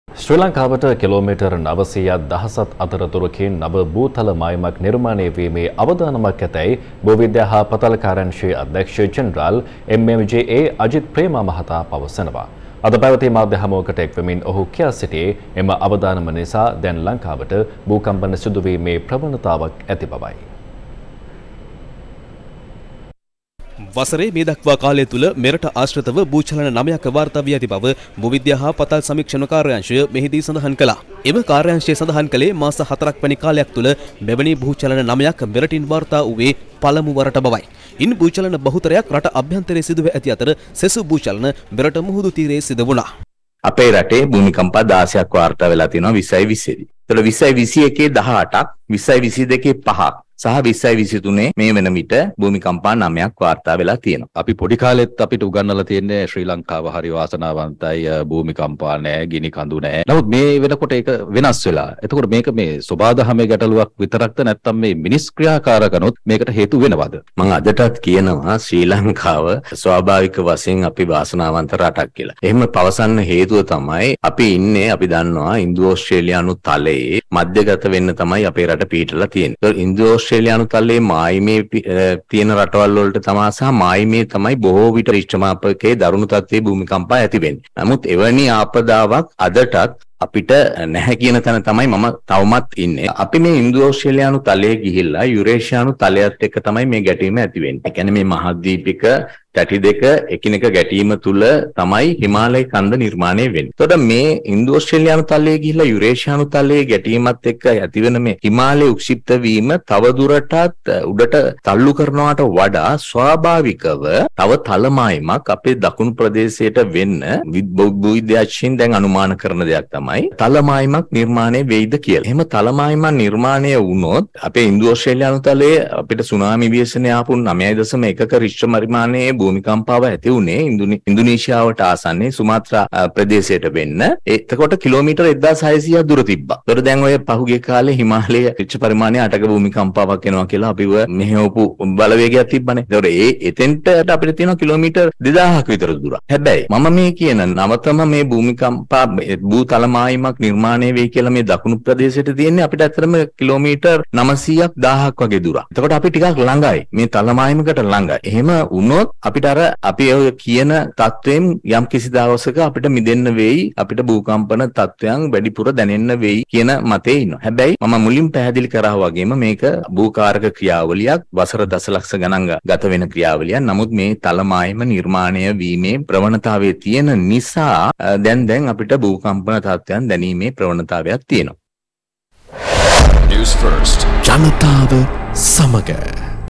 වෙනදට වඩා හෙල්ලුම්කන ලක් පොළොව.... මාස 4 යි.. භූ චලන 9 යි.. මෙහෙම වෙන්නේ ඇයි ?
අපේ දකුණට වෙන්න තවත් තල මායිමක් ? එහෙම වුණොත් කුමක් වෙයි ද?